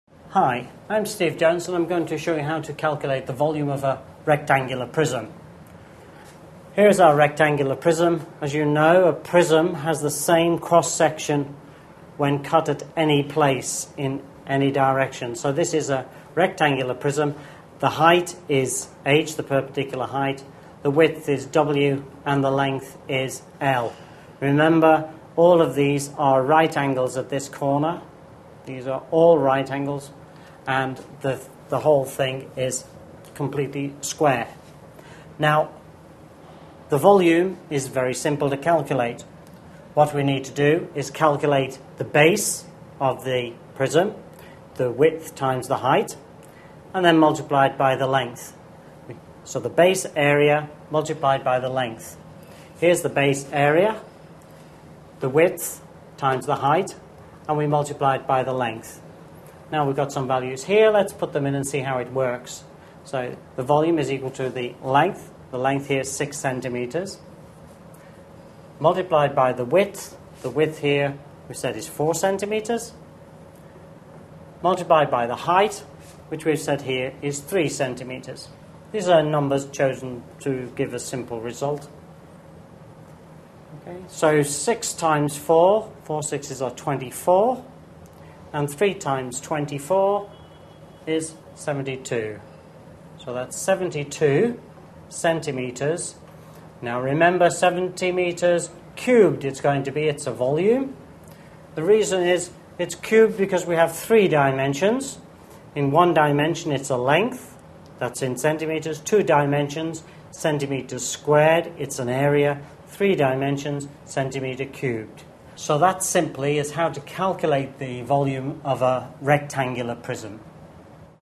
How to Calculate Volume of a Rectangular Prism
How to Calculate Volume of a Rectangular Prism. Part of the series: Mathematics & Science. Calculate the volume of a rectangular prism by measuring the base height and width, and multiplying it by the length. Determine the volume of a rectangular prism with tips from a teacher in this free video on math and science.